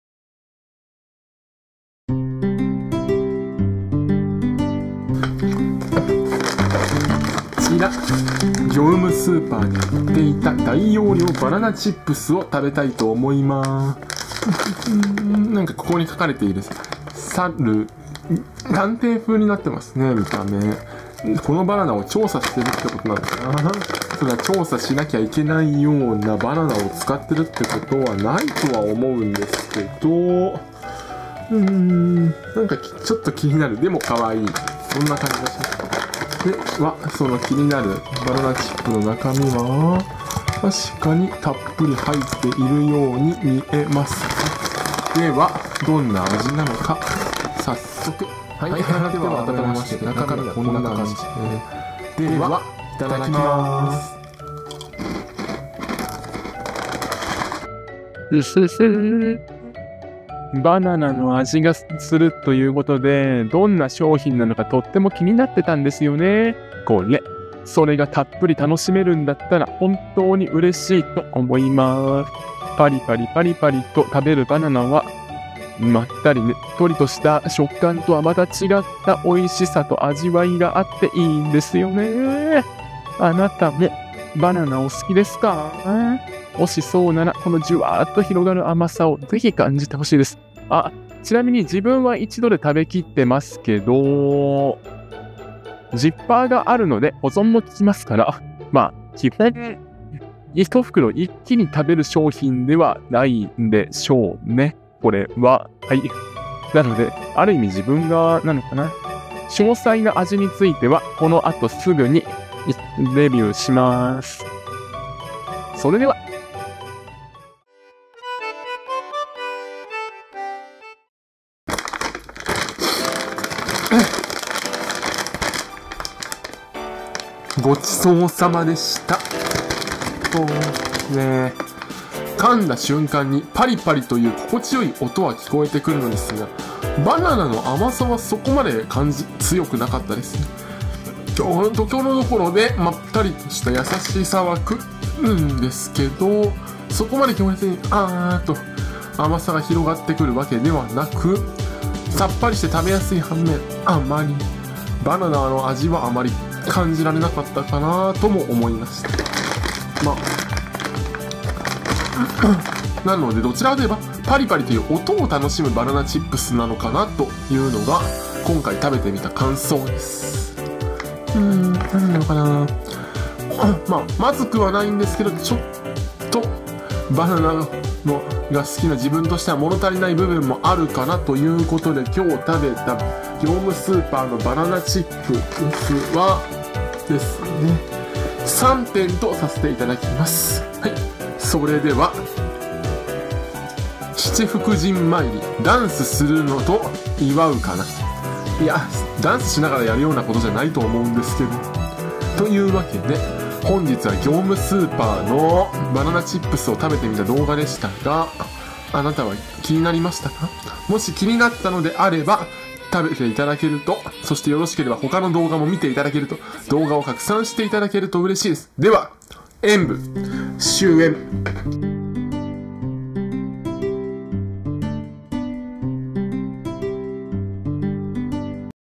【大食い】業務スーパーの大容量バナナチップスで、あなたもまったりしませんか
【まったり出来るチップスたっぷり!!】
業務スーパーで売られていた、
たっぷり大容量のバナナチップスを
かむと広がる優しい甘さで、
あなたもまったりしませんか?

アマゾンほしい物リスト↓